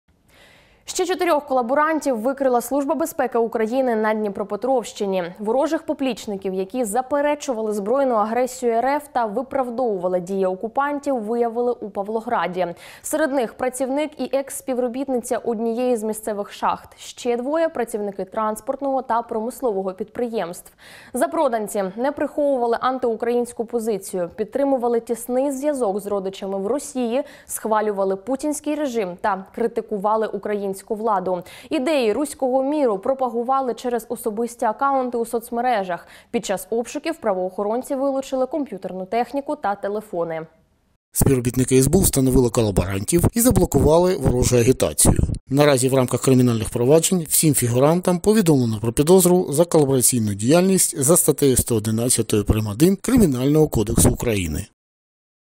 Чотирьох колаборантів викрила Служба безпеки України на Дніпропетровщині
Ще чотирьох колаборантів викрила Служба безпеки України на Дніпропетровщині. Ворожих поплічників, які заперечували збройну агресію рф та виправдовували дії окупантів, виявили у Павлограді.